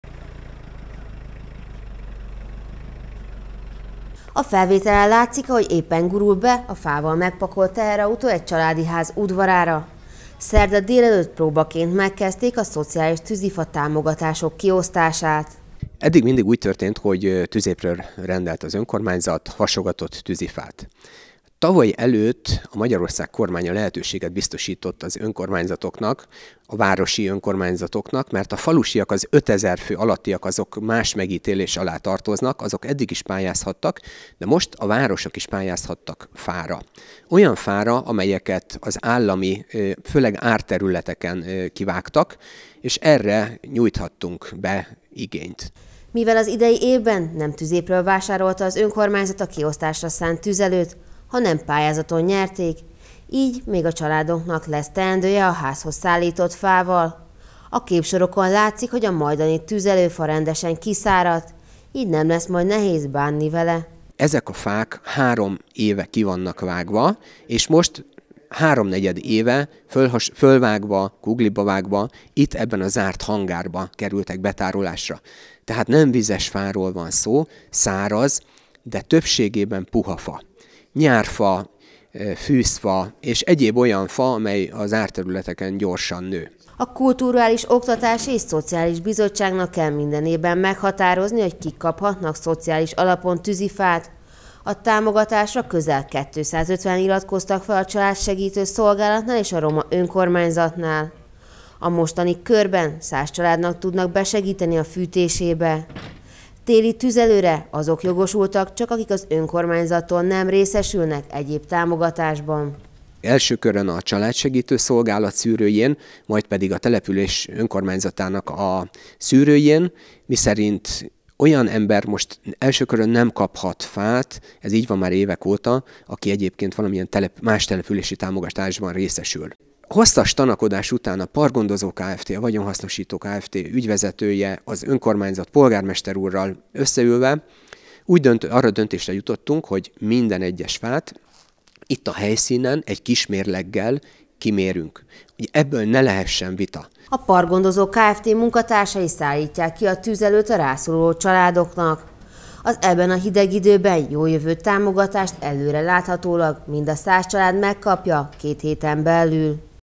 Megkezdődött a szociális tűzifa kiosztás – 100 hátrányos helyzetű családnak jut a támogatásból
Próbaként szerdán az első pár kiszállítás már megtörtént, de hivatalosan hétfőtől kezdődik meg a szociális alapú tűzifa segély kiosztása. Ezúttal 100 család részesül a támogatásban, háztartásonként 5 mázsa fát szállítanak házhoz a Parkgondozó Kft. munkatársai.